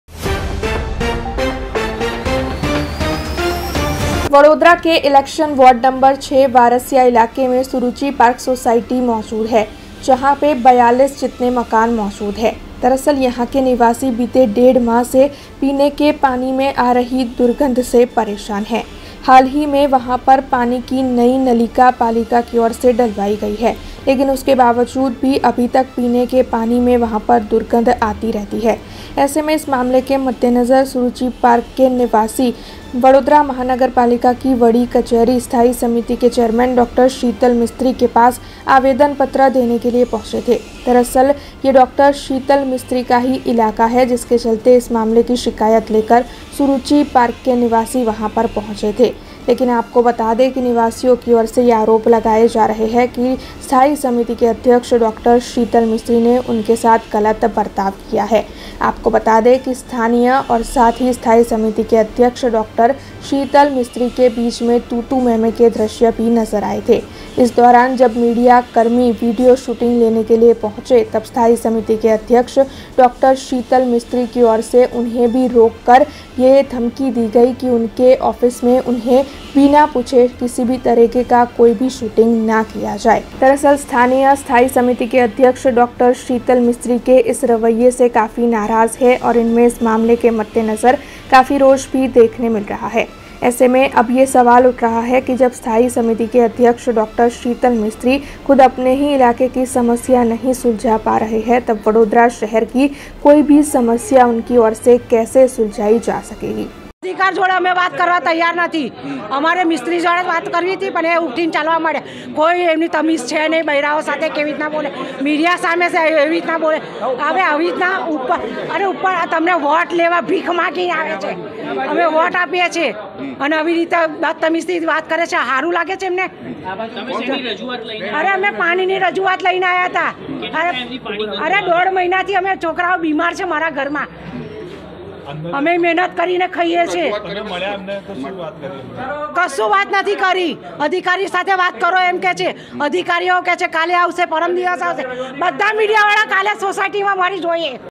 Anger among the residents of Suruchi Park Society due to the bad behavior of Dr Sheetal Mistry
BRG NEWS  App: